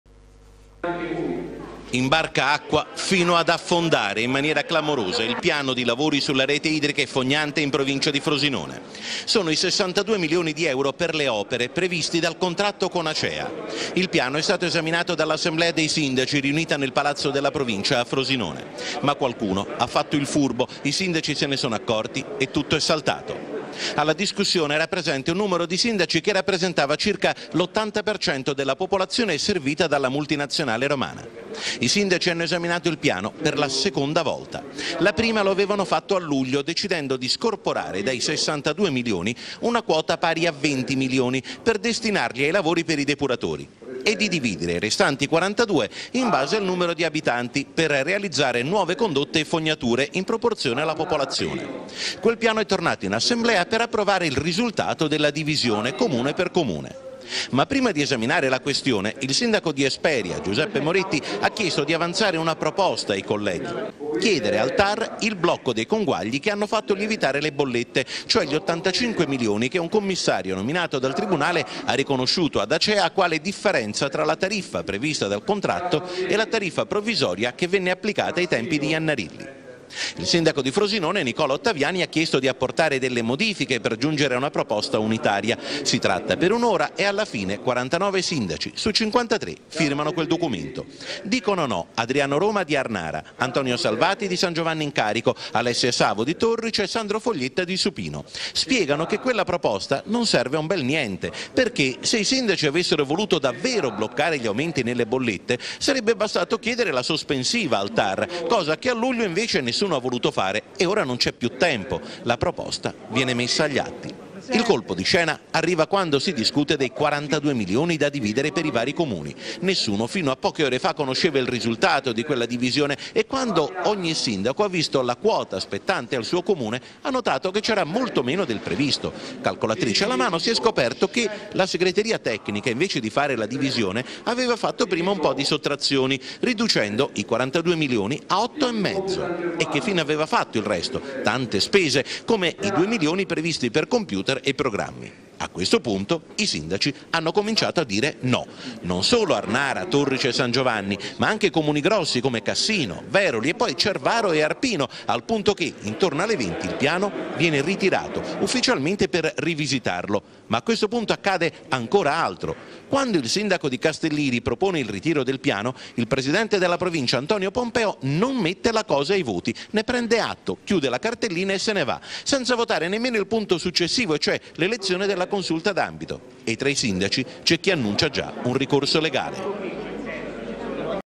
Conferenza dei Sindaci 9 Marzo 2015 - Acea Ato5 Fr
Imbarca acqua fino ad affondare in maniera clamorosa il piano di  lavori sulla rete idrica e fognante in provincia di Frosinone. Qualcuno ha fatto il furbo: i Sindaci se ne sono accorti e tutto è saltato.

Attorno alle 20 il Sindaco di Castelliri propone di ritirare il piano, che si era dimostrato fallimentare nel pomeriggio. Il presidente della provincia Pompeo non mette la proposta ai voti, prende la cartellina e se ne va. Senza nemmeno votare il punto successivo: il rinnovo della consulta d'ambito.